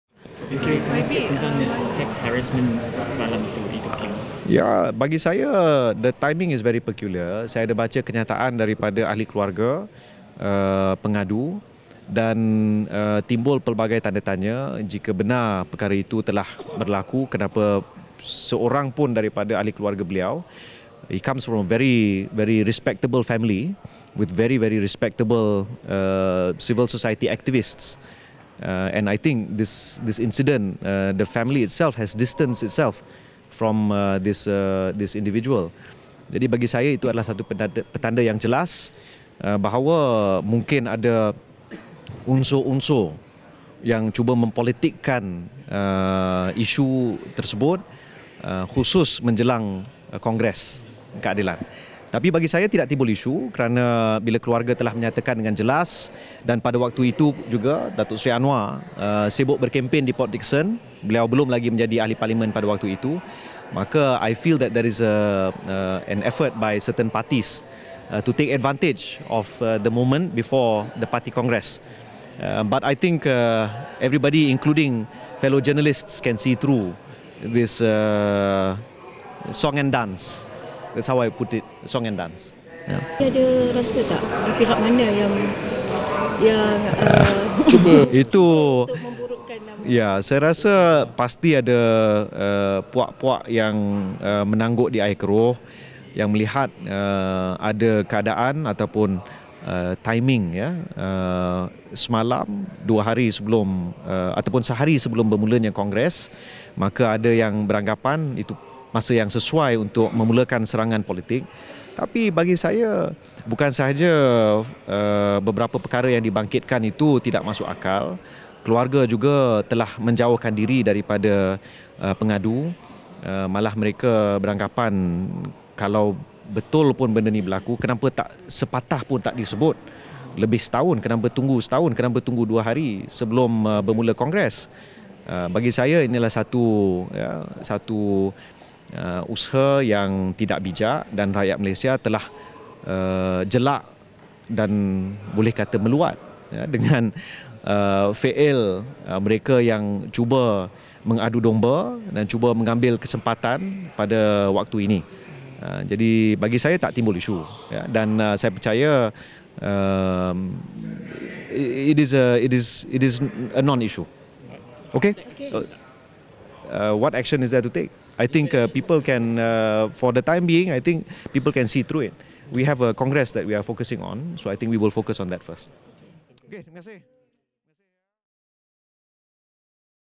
Fahmi: Timing of sex claim is peculiar
PKR communications director Fahmi Fadzil said the timing of a statutory declaration containing startling sexual harassment allegations involving a top PKR politician and a 26-year-old was peculiar.

The statutory declaration made by the alleged victim, Muhammed Yusoff Rawther, claimed PKR president Datuk Seri Anwar Ibrahim had attempted to coerce him into oral and anal sex in the leader’s office.

Fahmi added that since Yusoff’s family member came forward to dispute his claim, it was a clear indication that a political ploy was involved.